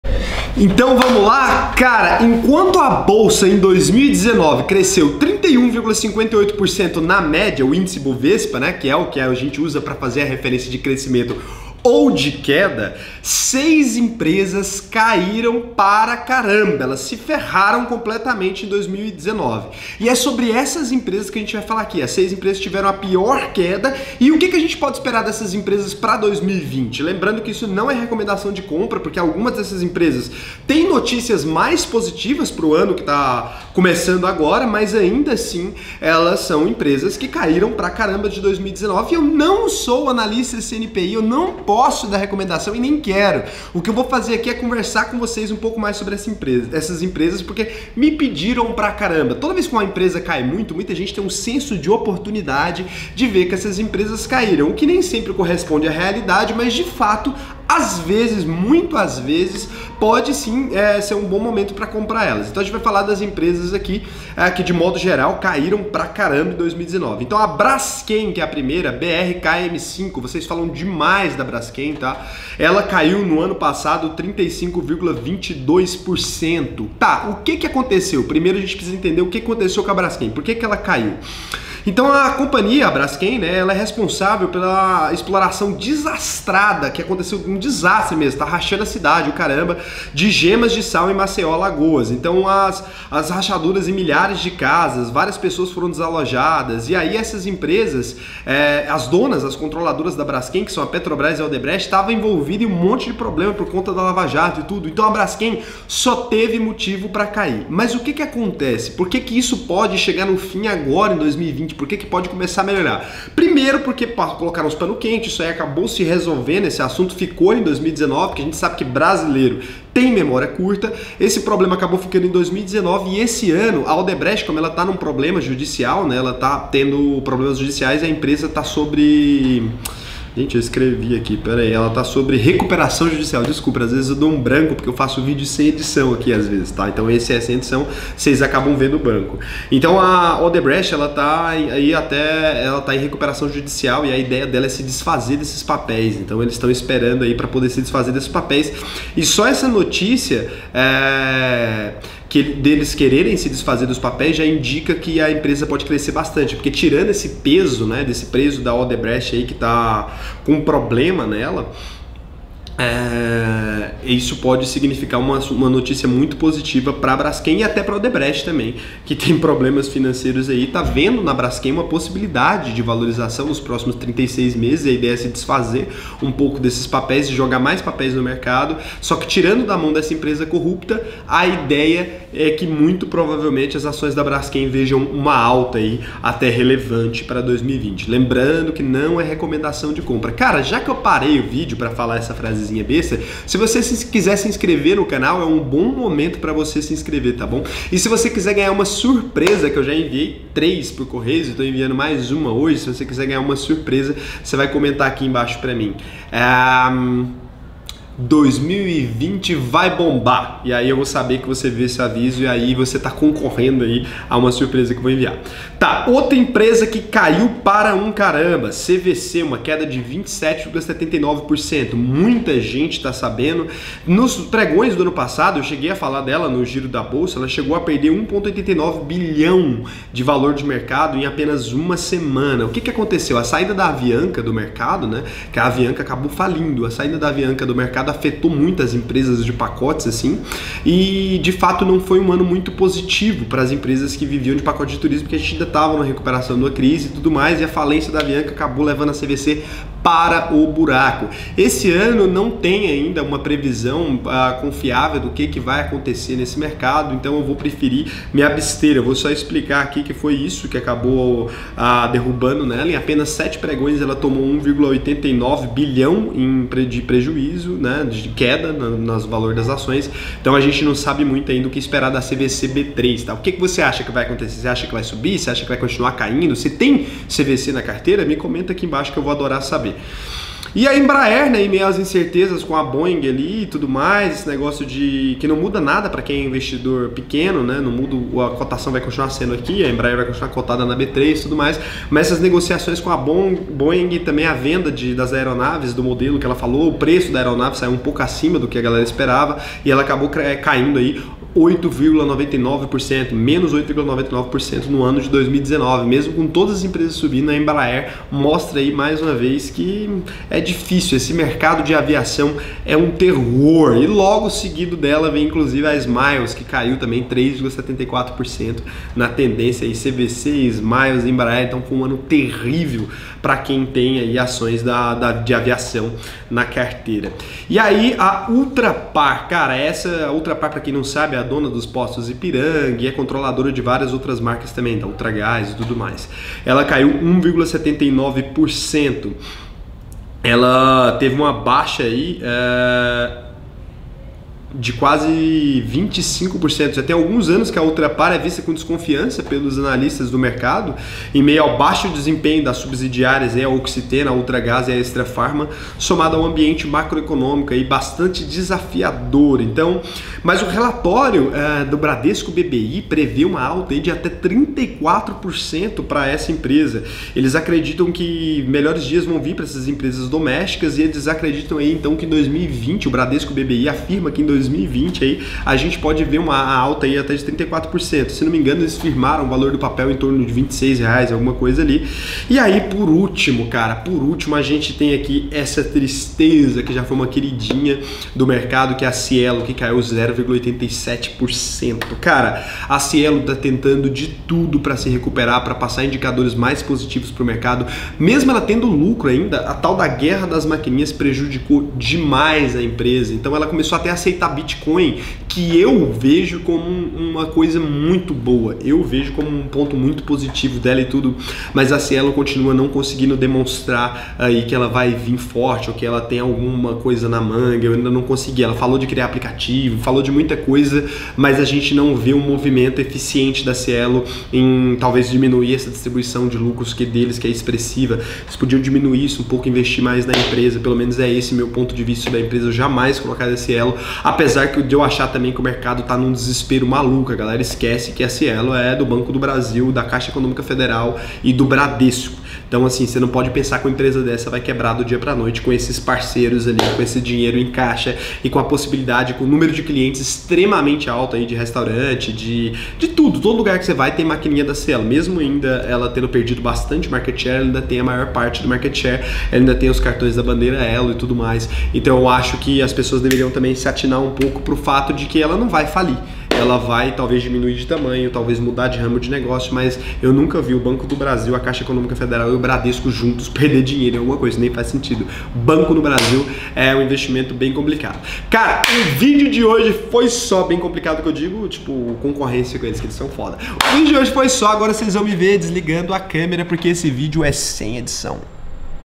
VALE A PENA INVESTIR NAS 6 PIORES EMPRESAS DE 2019 PARA 2020?
As lanterninhas de 2019, as empresas que mais caíram em 2019, será que elas vão ter um resultado melhor em 2020? Nosso papo de hoje é sobre isso.

Papel Código Variação (%)
BRASKEM PNA BRKM5 -35,22